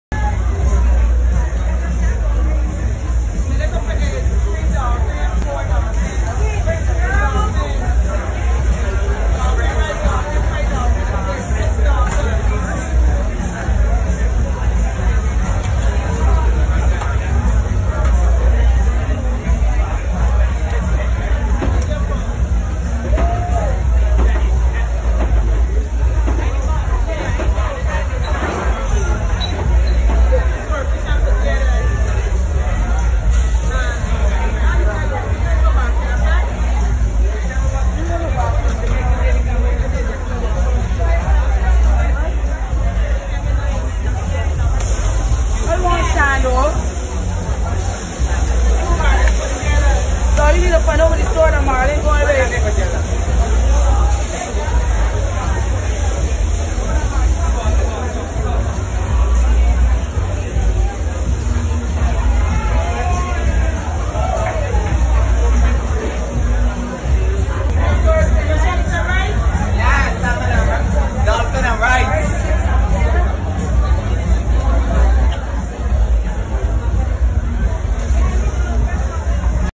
Oistins Fish Fry BBQ may 2019
Uncly Georges fish fry- A real fisherman - He Know how to trim the fish so it all lean - More like this